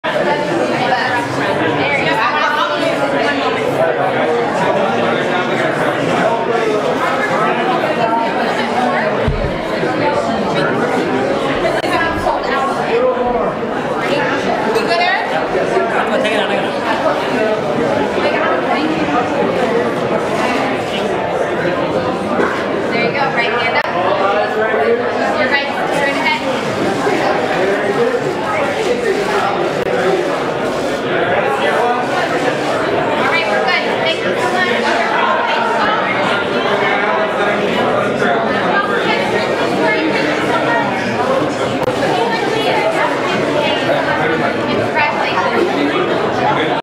First Somali-American elected to Congress sworn in
Rep. Ilhan Omar, a Minnesota Democrat, was officially sworn in as a member of the 116th Congress, becoming the first Somali-American elected to the House of Representatives.